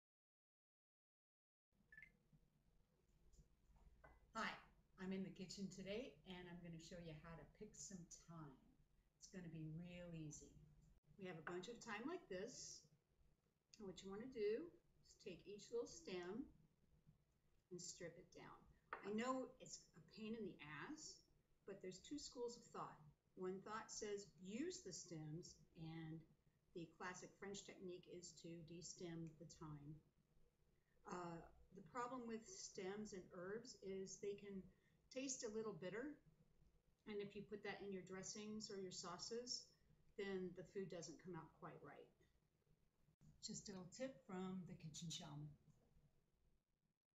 Picking Thyme
The Kitchen Shaman shows you how to pick thyme.

Where to find more FOOD FUN & FACTS